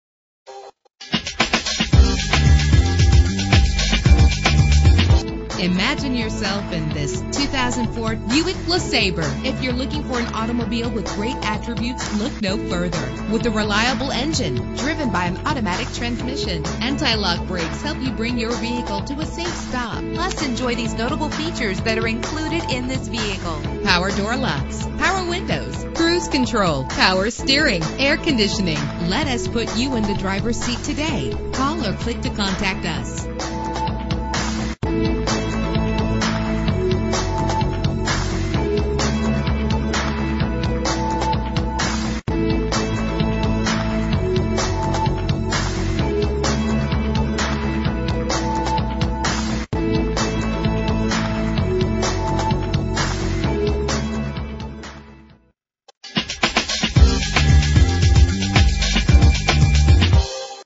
2004 Buick LeSabre - Kokomo IN
Year: 2004
Make: Buick
Model: LeSabre
Trim: Custom Sedan 4D
Engine: 6 cylinder 3.8 Liter
Transmission:
Color: Blue
Mileage: 97314
Address:
1220 East Boulevard